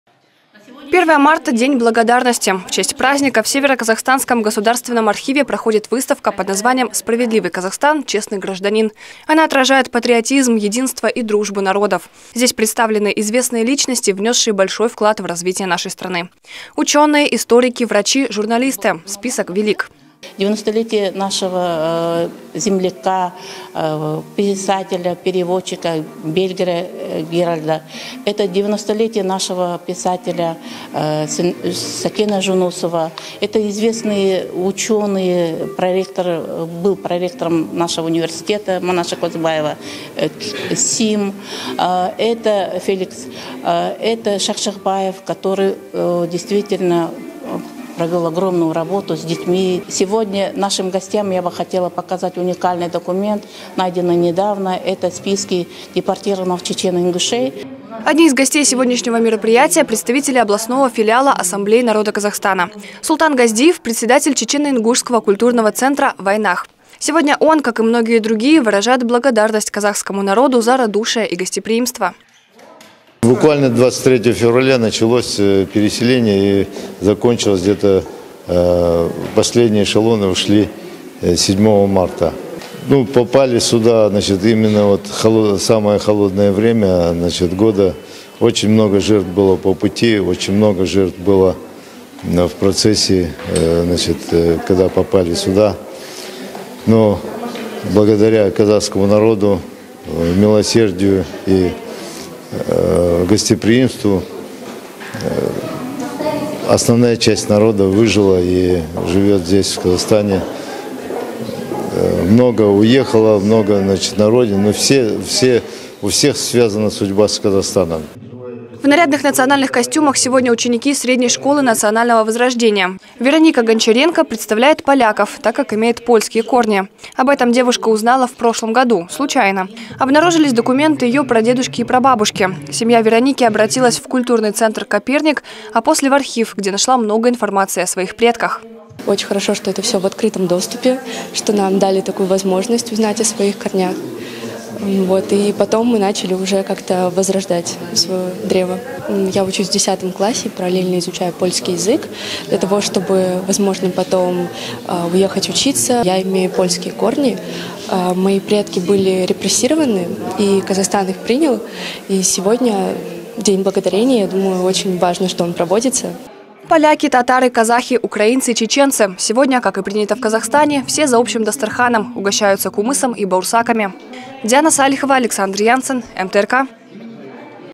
День благодарности отмечают в государственном архиве СКО
1 марта - День благодарности. В честь праздника в Северо-Казахстанском государственном архиве проходит выставка под названием «Справедливый Казахстан - честный гражданин».